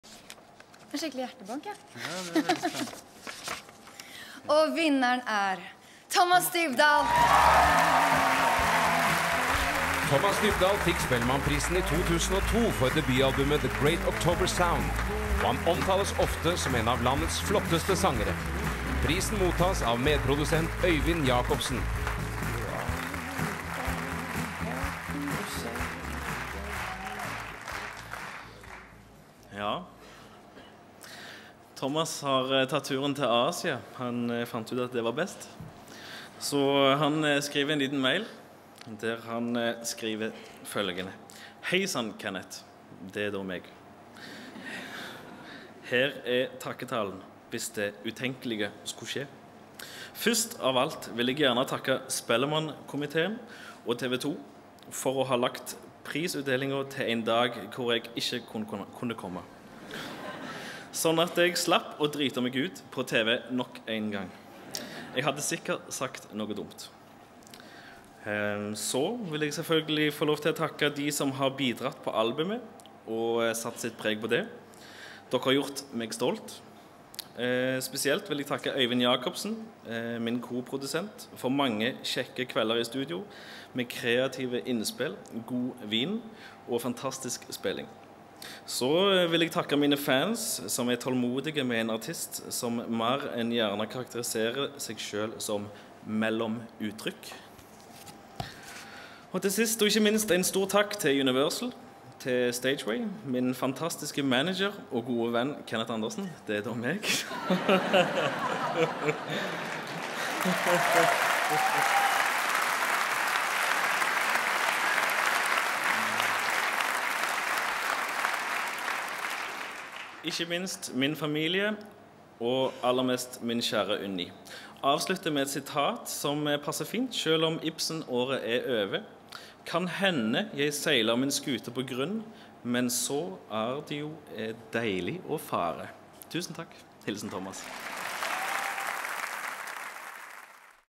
Thomas Dybdahl vinner Årets Mannlige Artist (Spellemannprisen 2006)
Thomas Dybdahl vinner prisen for Årets Mannlige Artist 2006